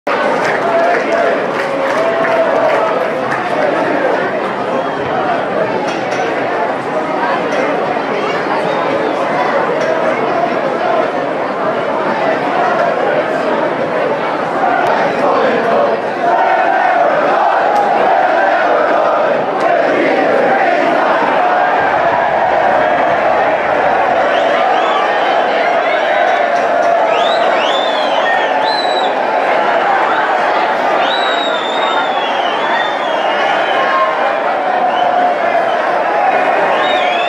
Goal: Jamie Lennon (vs Shamrock Rovers 09/02/2024)
Jamie Lennon scores a late consolation goal for the Saints in the President's Cup Final against Shamrock Rovers at the Tallaght Stadium.